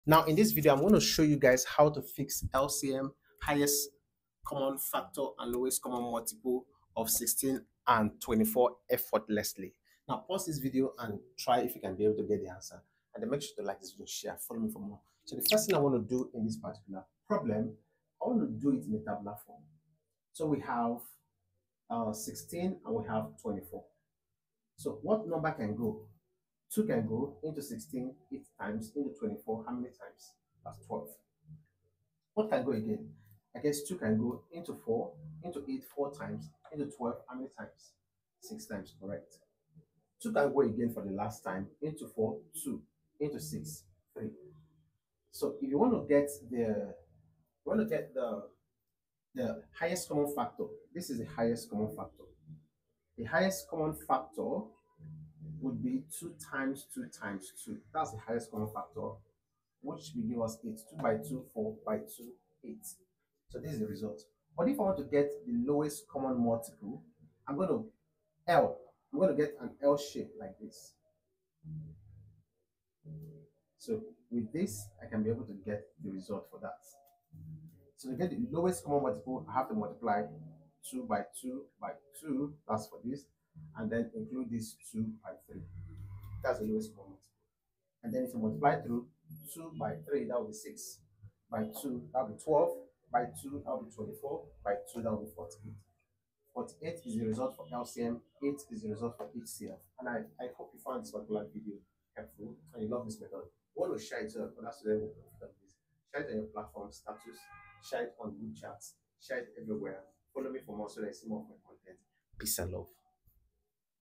A smarter way to fix the HCF and LCM of 16 and 24
Quick Trick to Find HCF and LCM Easily!

In this video, I explained a smart and time-saving method to find both the HCF (Highest Common Factor) and LCM (Lowest Common Multiple) of two numbers.

I started by showing the normal process of finding the HCF step by step, then introduced a simple “L-shape” trick, a visual method that helps you move smoothly from HCF down to LCM in one tabular form.

This approach not only makes the calculation faster but also helps students understand the relationship between HCF and LCM clearly.

Whether you’re preparing for school exams, WAEC, JAMB, GCSE, IGCSE, SAT, or GRE, this video will sharpen your math skills.

👉 Watch, learn, and share with friends who love solving math problems smartly!